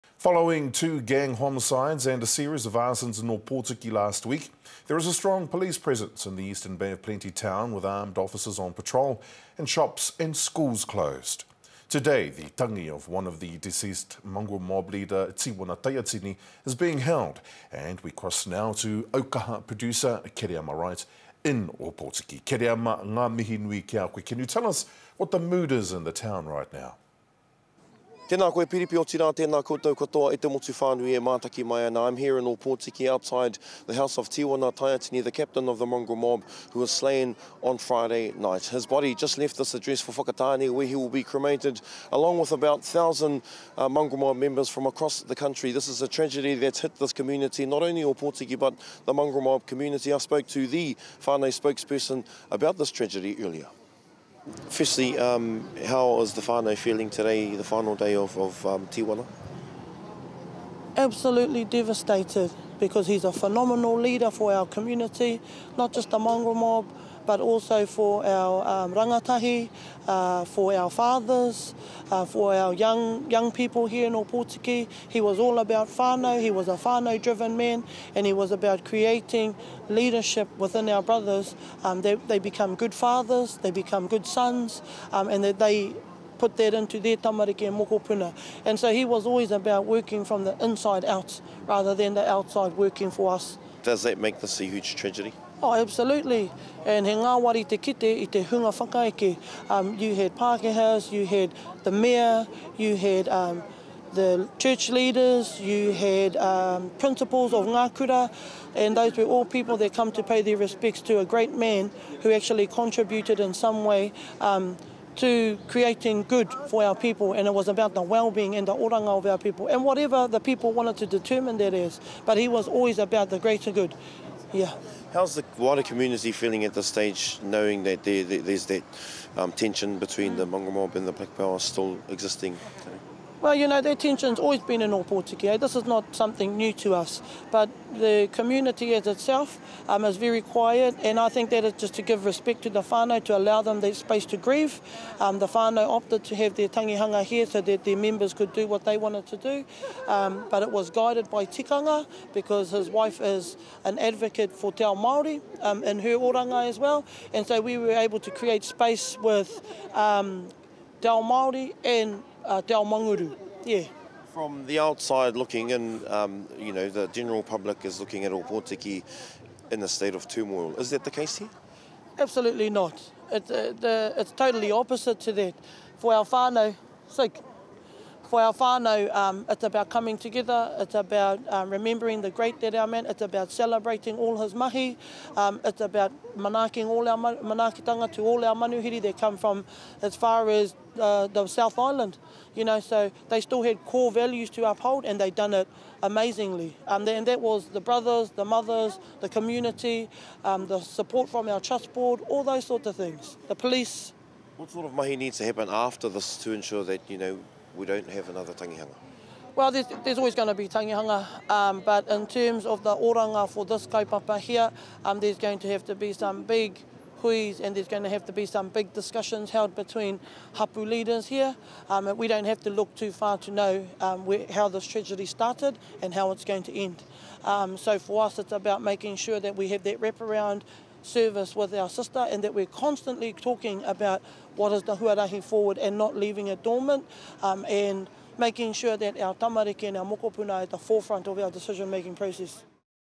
Strong police presence in Ōpōtiki following death of Mongrel Mob leader
The Bay of Plenty township of Ōpōtiki is under increased police presence as the tangi of deceased Mongrel Mob leader Tiwana Taiatini proceeds today.